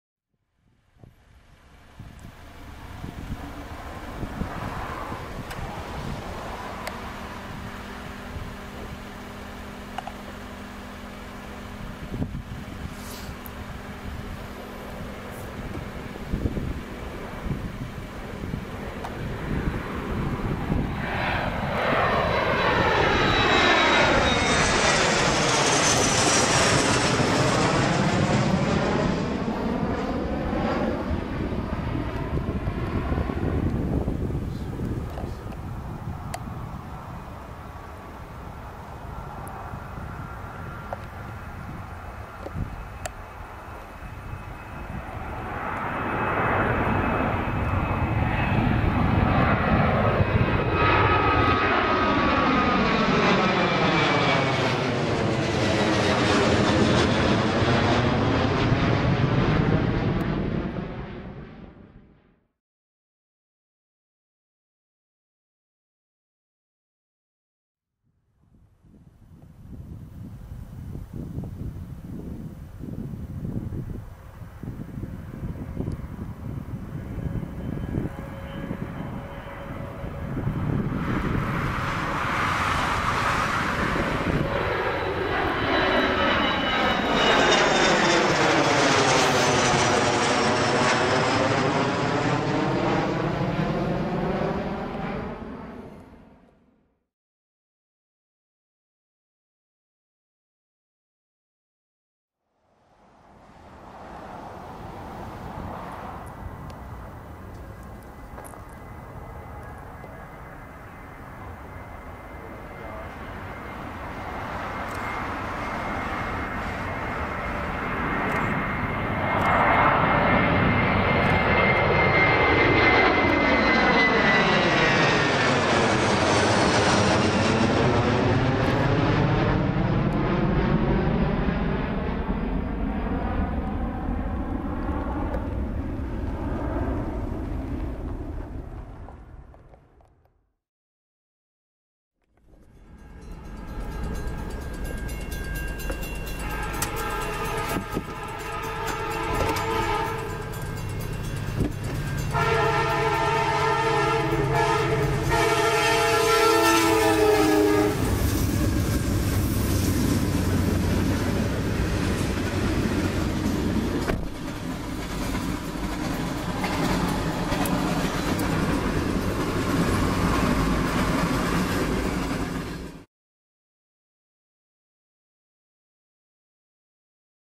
Plane Spotting IAH 10/31/2010
On a sunny Halloween afternoon, Dad and I went out to the Houston Intercontinental airport to do some plane spotting. The planes were taking off at the normal plane spotting area instead of the usual landing... which was a bad thing because I kept losing focus on the planes in my videos.

Later while going home in Kingwood, we caught a fast UP freight with a nice lashup and a powerful horn.